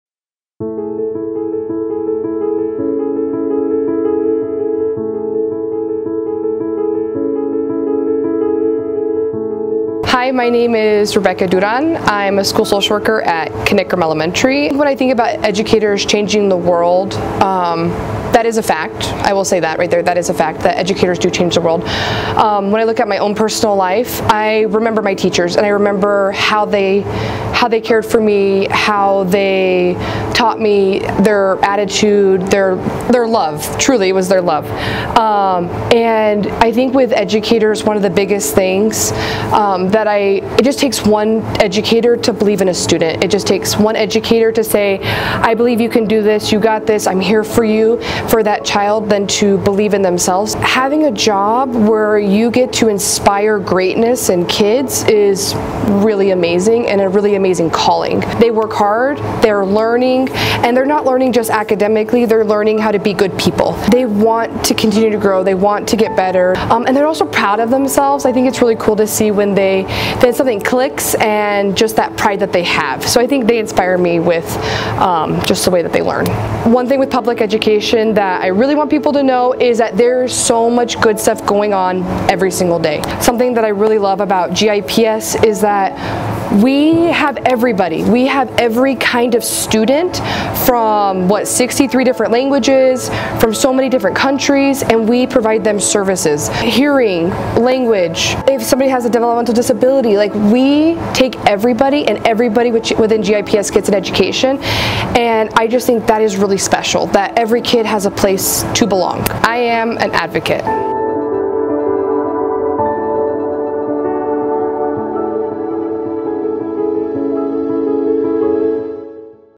We Are GIPS - Rebecca Duran Meyer, Social Worker
Mrs. Meyer is a Social Worker at Knickrehm Elementary.

She believes in the power of public education.  Here she shares how educators help students believe in themselves and inspire greatness.

Because We Are GIPS.